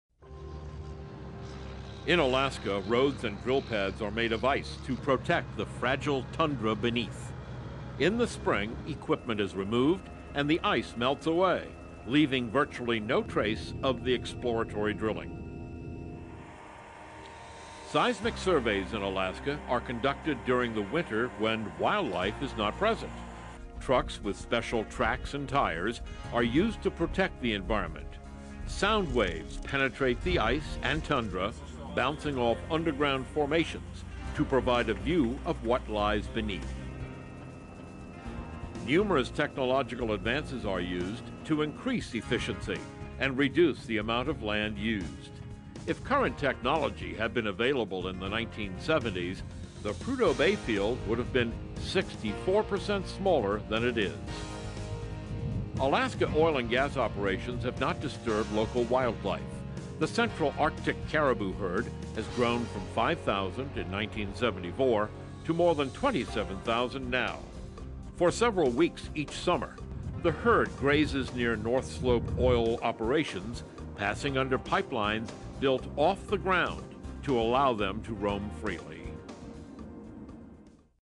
Arctic Technology
Specialized technology enables development in Arctic environments.
Courtesy American Petroleum Institute (API)